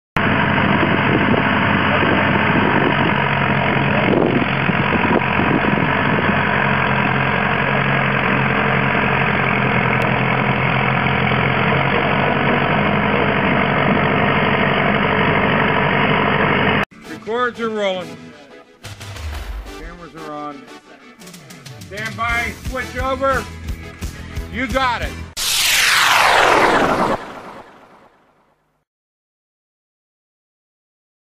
Leveling the playing field
New Wilmington, PA-- Construction continues on the artificial field of dreams for Bury Stadium at Westminster College (shot May 2008).